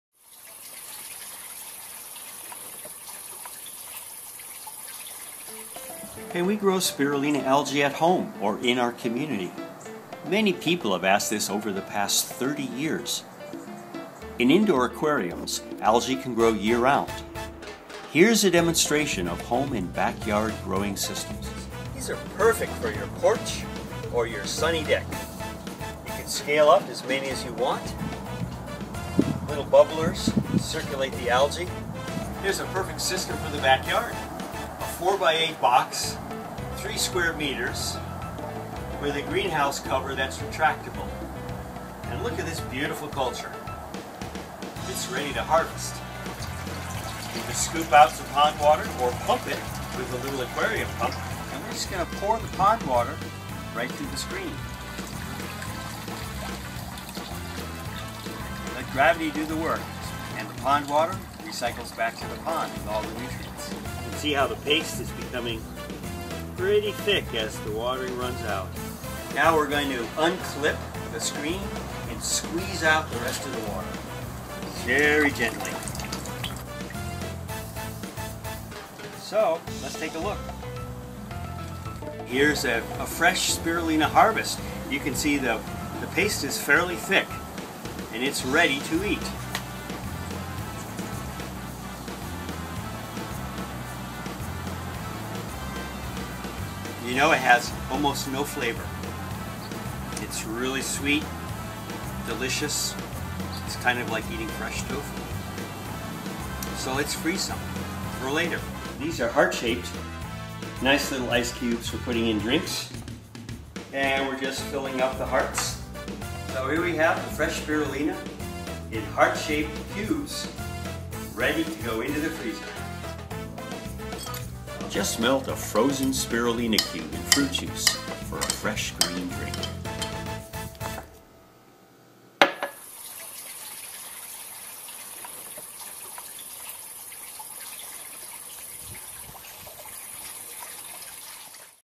Robert Henrikson • Growing Spirulina Home and Backyard 2012 • Spirulina Voices
Spirulina Voices: Robert Henrikson shows growing spirulina at home in aquarium tanks and in the backyard with a 4x6 covered pond, harvesting fresh spirulina, freezing and adding to drinks. Richmond California, 2012. Produced by Robert Henrikson.